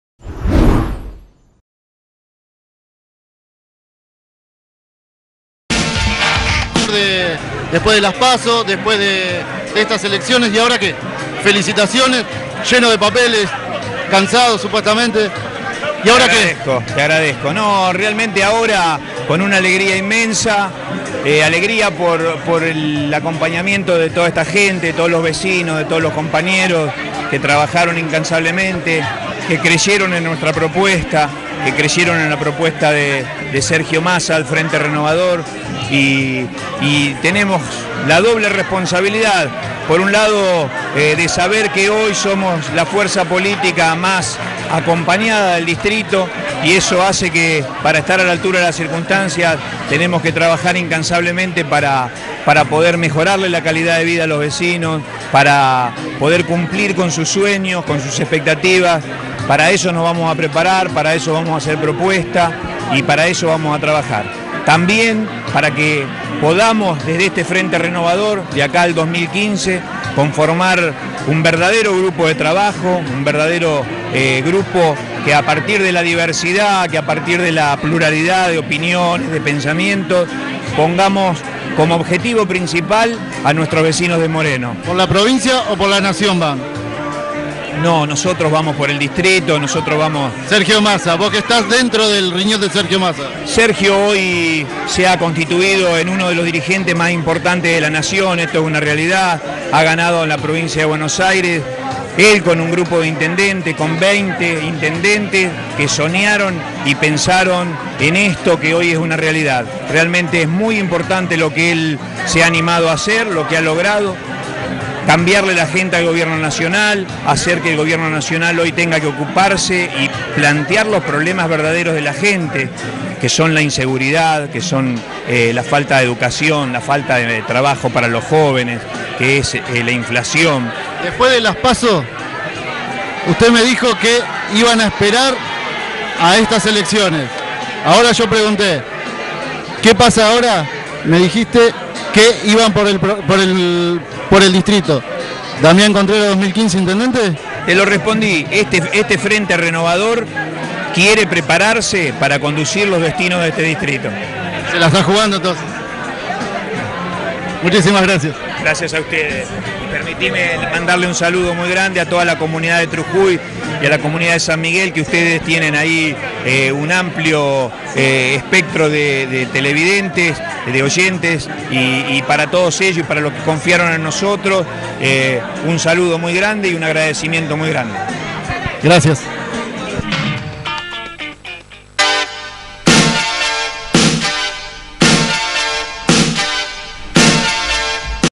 GANO DAMIAN CONTRERAS EN MORENO
El Frente Renovador gano en Moreno con 10 puntos de diferencia
Damián Contrera se alzo con un  triunfo contundente